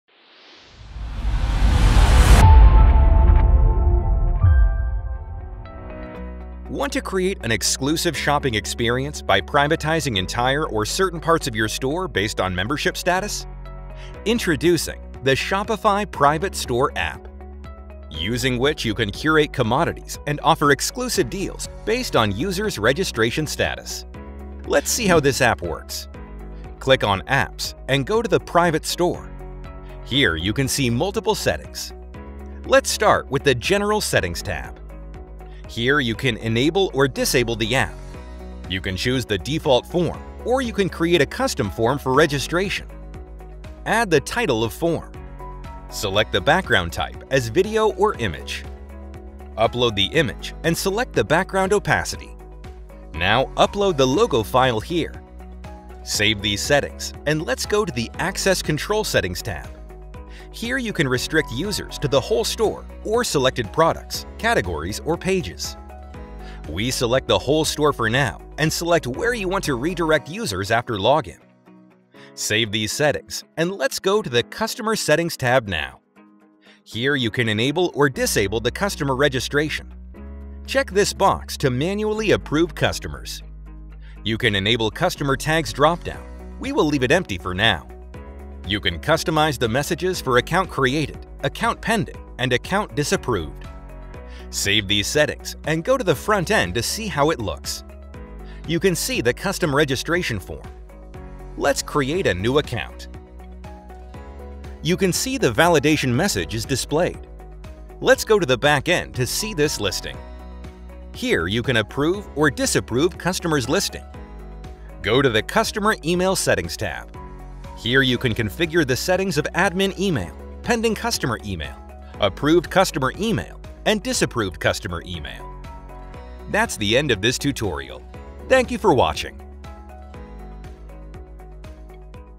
Memberly ‑ Private Store - Addify
Private Store App enables you hide store or specific products, collections and pages from non-logged in users. Manually review and validate new customer registrations to build a curated community and offer them exclusives products and services. B2B solution to create an exclusive members-only store or restricted members-only products or pages in your store. Use your standard login/registration form or custom login/registration form with an option to add image or video in background.